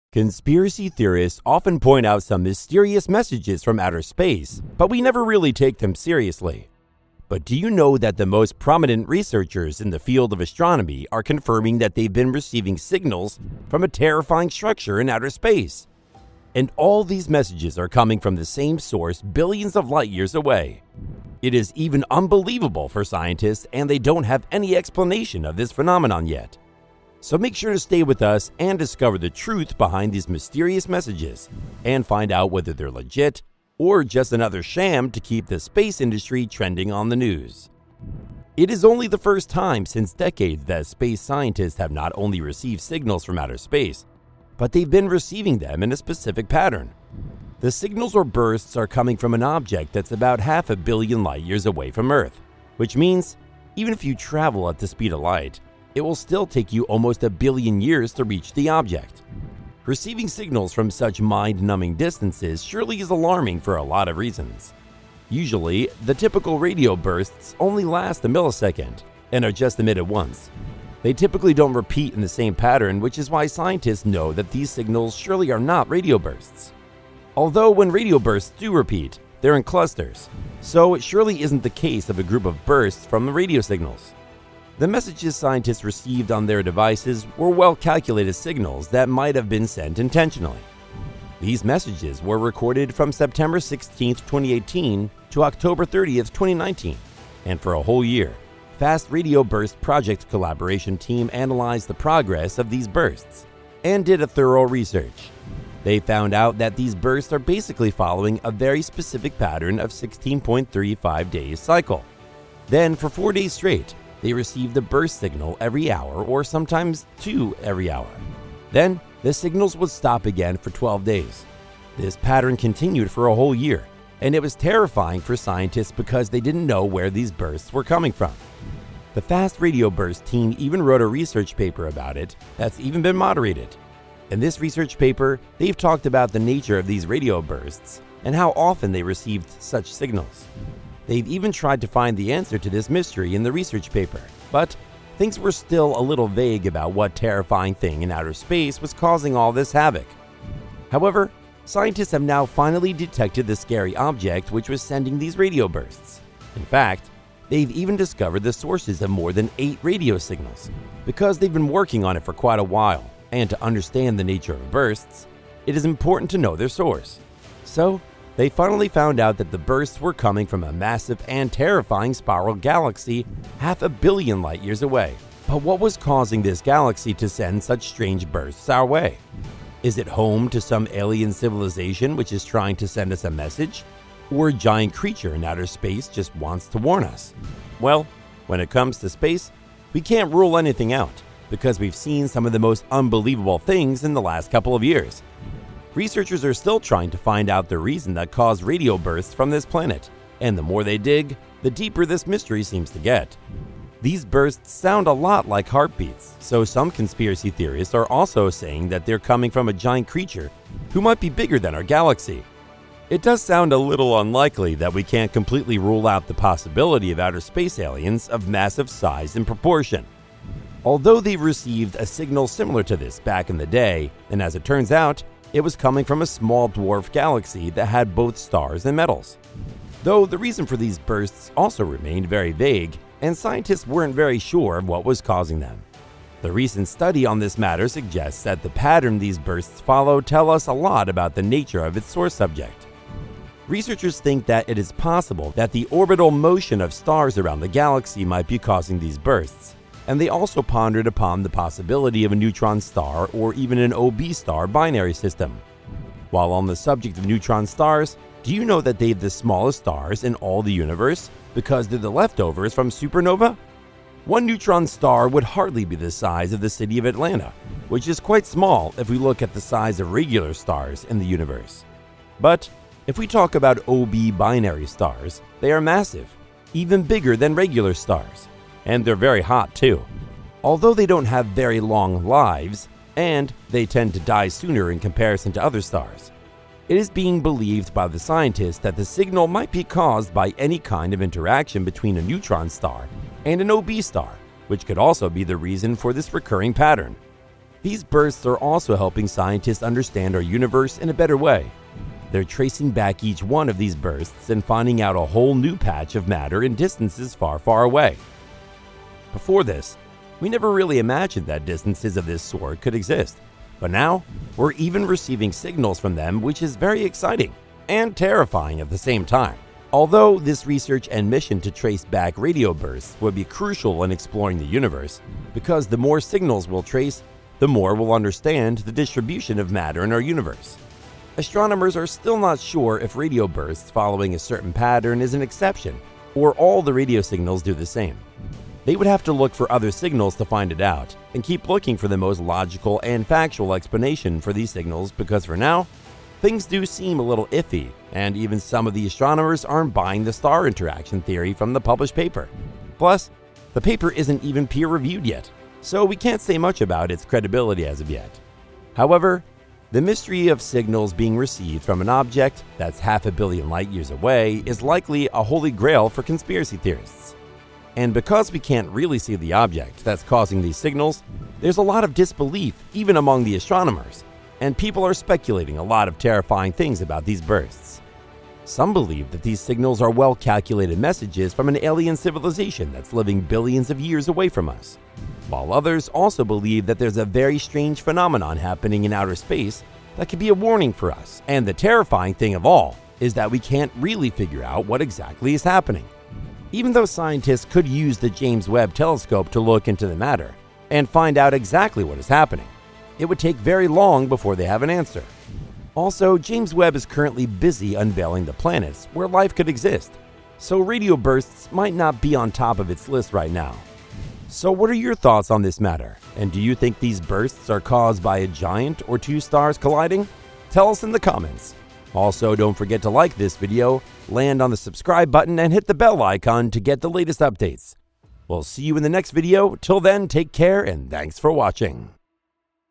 Scientists Just Detected A TERRIFYING Structure That Keeps Sending Us Messages!
🚀 For more Space Related Content!

Conspiracy theorists often point out some mysterious messages from outer space but we never really take them seriously. But do you know that the most prominent researchers in the field of astronomy are confirming that they’ve been receiving signals from a terrifying structure in outer space. And all these messages are coming from the same source billions of light years away.

It is even unbelievable for scientists and they don’t have any explanation of this phenomenon yet. So, make sure to stay with us and discover the truth behind these mysterious messages and find out whether they’re legit or just another sham to keep the space industry trending on the news.

It is only the first time since decades that space scientists have not only received signals from outer space but they’ve been receiving them in a specific pattern. The signals or bursts are coming from an object that’s about half a billion light years away from earth which means, even if you travel at the speed of light, it will still take you almost a billion years to reach the object. Receiving signals from such mind-numbing distances surely is alarming for a lot of reasons.

Usually, the typical radio bursts only last a millisecond and are just emitted once. They typically don’t repeat in the same pattern which is why scientists know that these signals surely are not radio bursts. Although, when radio bursts do repeat, they’re in clusters so, it surely isn’t the case of a group of bursts from radio signals. The messages scientists received on their devices were well-calculated signals that might have been sent intentionally.